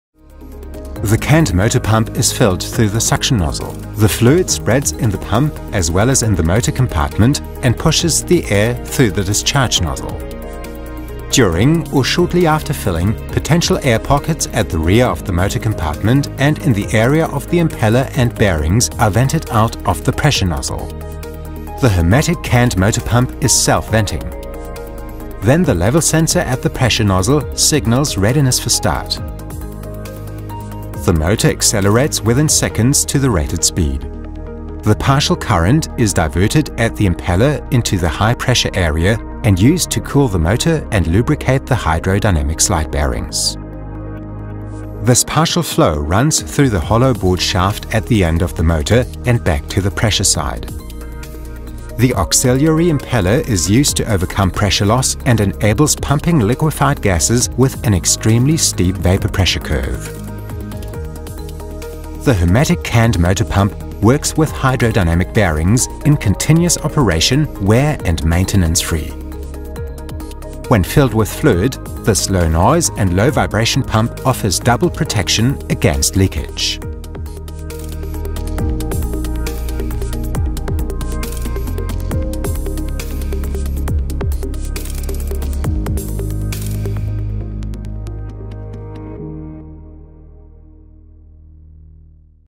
HERMETIC canned motor pump type CNF
The CNF model is the version for liquefied gases, boiling media and condensate.
The integrated fluid return and HERMETIC's own design with internal auxiliary wheel allow installation without additional piping and reliably protect against internal cavitation.